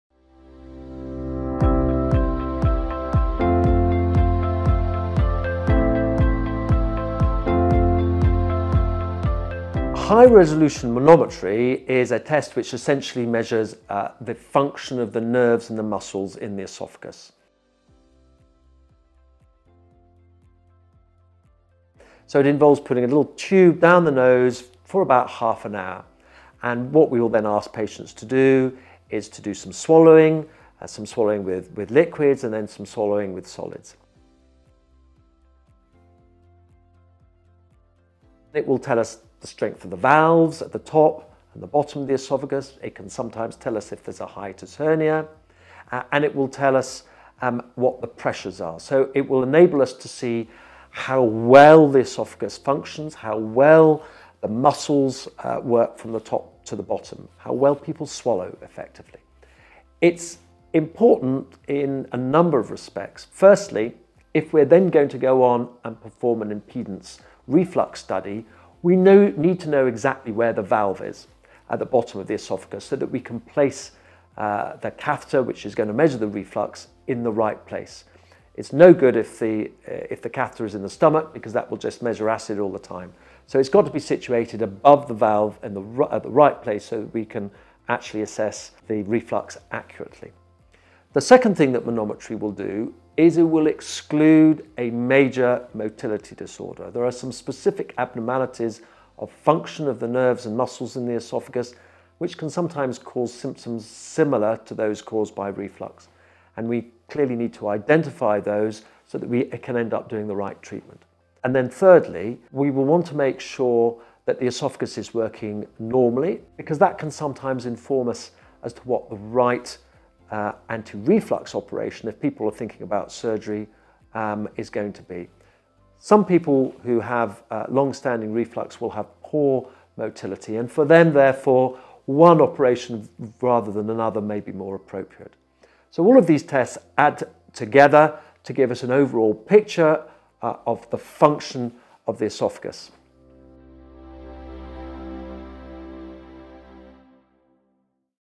What is a HRM (high resolution manometry) test?
Mr Boyle Medical Director RefluxUK and Upper GI surgeon explains what a HRM test is looking for and how it is performed.

To find out more about High-Resolution Manometry and other diagnostic tests for reflux, please click this link: https